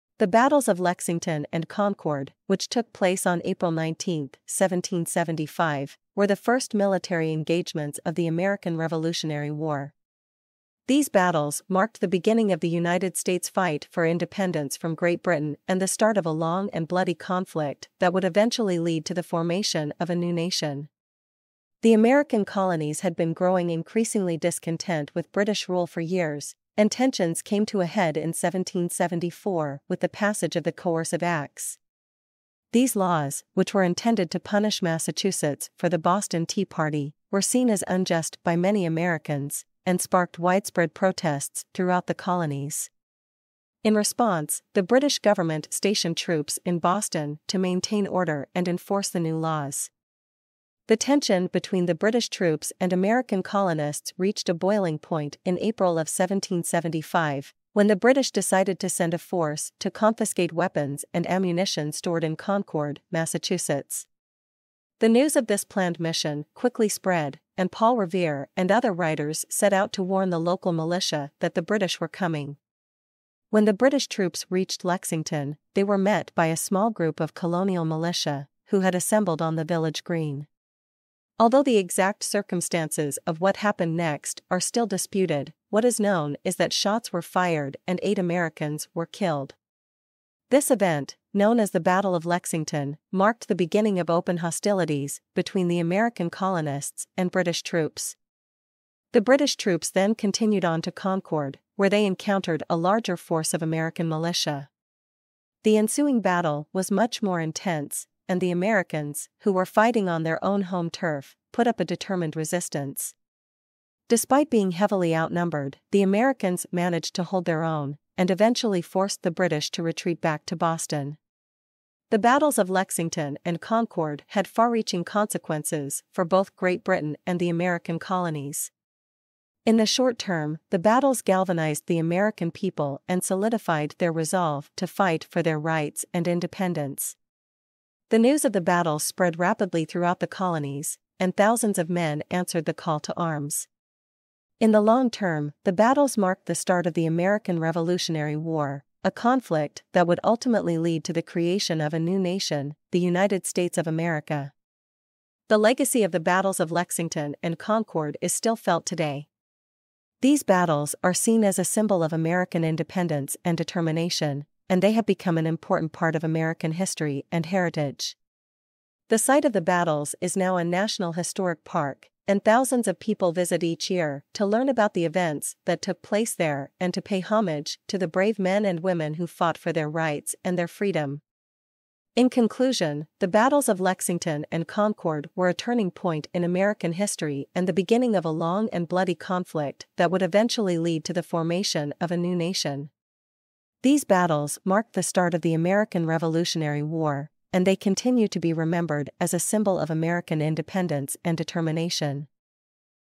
The Battles of Lexington and Concord
The Battles of Lexington and Concord were the first military engagements of the American Revolutionary War, which took place on April 19, 1775. The conflict arose from growing discontent with British rule and tensions came to a head with the passage of the Coercive Acts. When the British attempted to confiscate weapons and ammunition in Concord, they were met with resistance from American militia, leading to the battles in Lexington and Concord. The battles galvanized the American people and solidified their resolve to fight for their rights and independence, and marked the beginning of the American Revolutionary War and the eventual formation of the United States. The battles continue to be remembered as a symbol of American independence and determination.